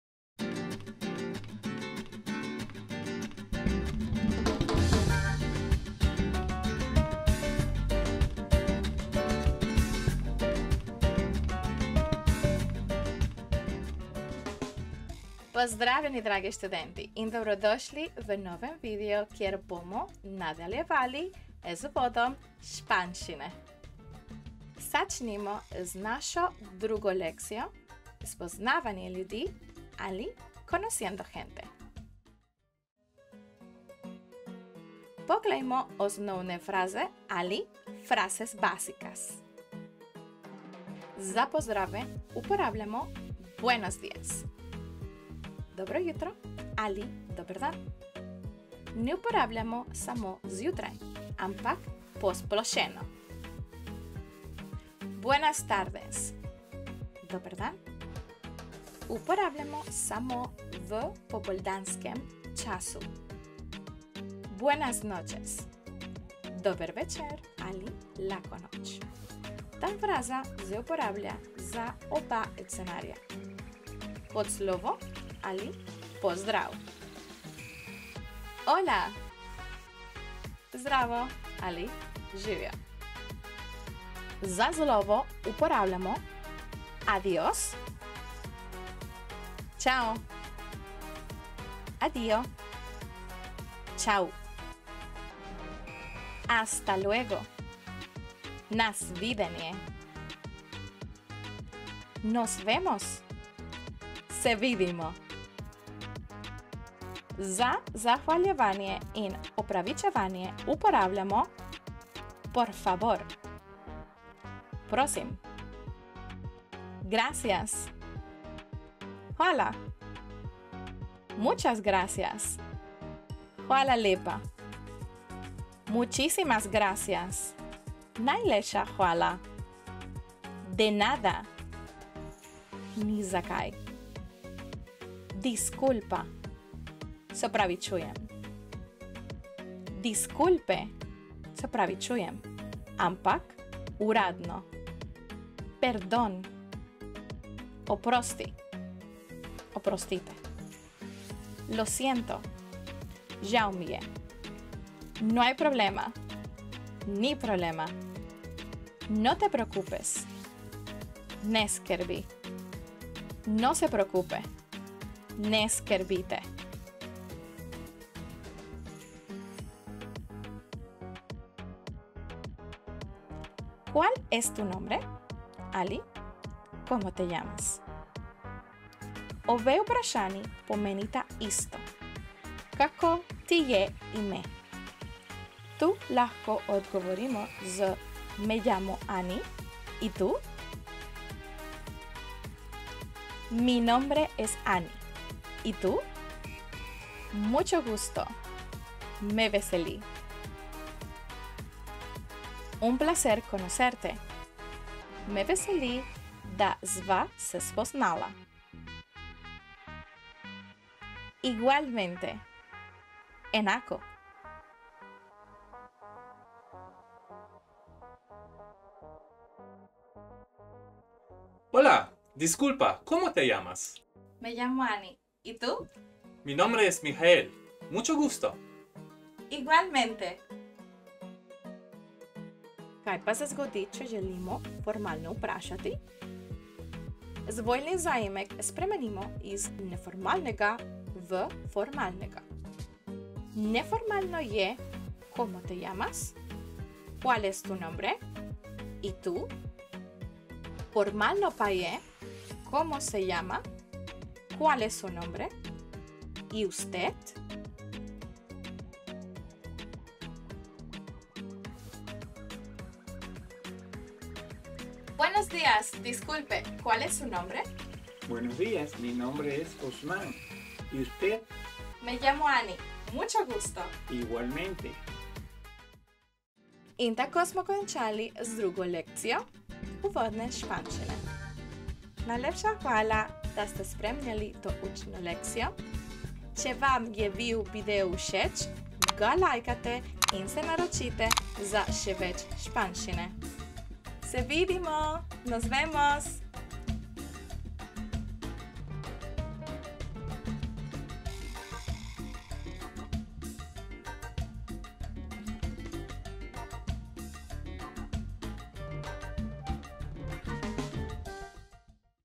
SPOZNAVANJE LJUDI - Uvodna Lekcija #2
🇸🇮  V tej drugi lekciji imamo osnovne fraze za pozdrave, slovo, zahvaljevanje in opravičevanje.
Če se boste naročili na kanal boste v prihodnjih tednih zasledili še več takih videev, kjer se boste lahko naučili osnov španščine.

Časovni zamiki:
0:00 - Intro
0:14 - Uvod
0:33 - Osnovne fraze
0:41 - Za pozdrave
1:24 - Za slovo
1:47 - Za zahvaljevanje in opravičevanje
2:48 - Osnovno se lahko predstavimo tako...
3:36 - Vaja
3:52 - Če želimo formalno vprašati
4:19 - Pa še povadimo
4:36 - Zaključek

Tukaj pa zdaj delim predogled knjige.

Knjiga bo v prihodnosti v celoti ali po delih na voljo na razpoložljivi spletni strani. Ko bo končana, boste obveščeni.

 🇪🇸 Vaša vodnica po Španščini - Annie.
💡 - Potrebujete pomoč pri učenju jezika? Kontaktirajte me